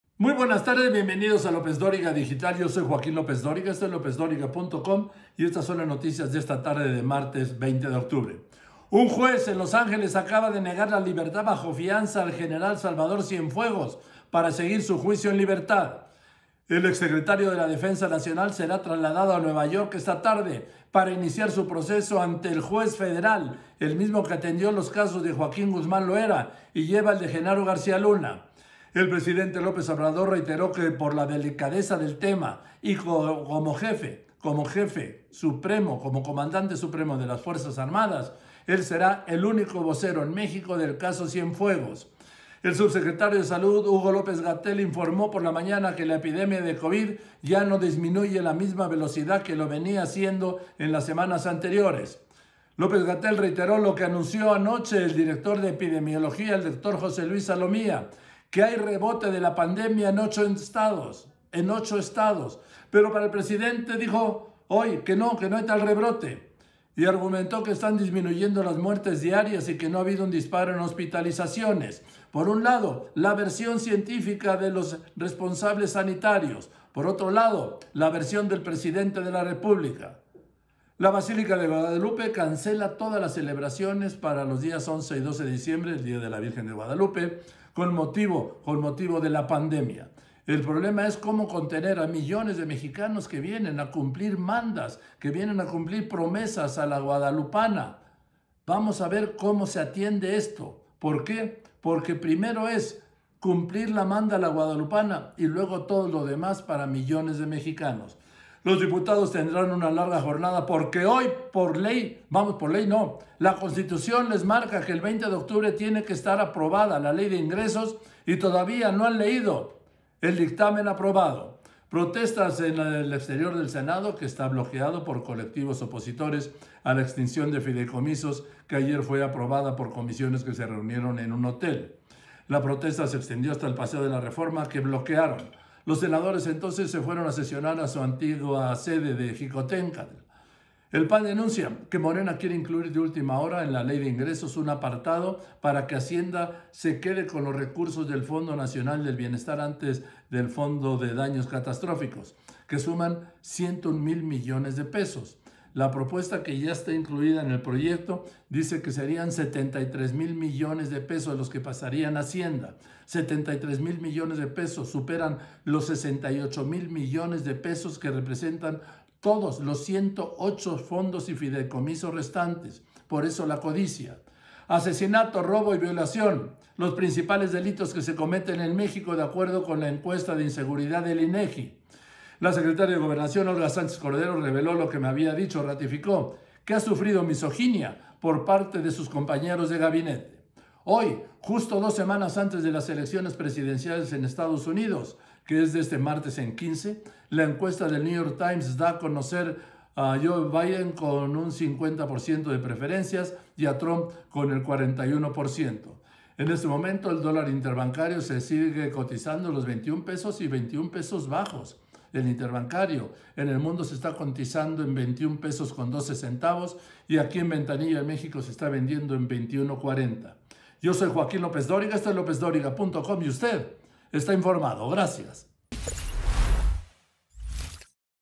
Niegan fianza al general Salvador Cienfuegos y será trasladado a Nueva York
¡Las Noticias!

Niegan fianza al general Salvador Cienfuegos y será trasladado a Nueva York; el presidente López Obrador, el vocero del caso; riesgo de rebrote en México; La Basílica de Guadalupe cancela todas sus celebraciones del 11 y 12 de diciembre; Diputados tendrán larga jornada por Ley de Ingresos, protestas en el Senado y más

Lo más importante que está pasando en México y en el mundo minuto a minuto. Cobertura total en López-Dóriga Digital.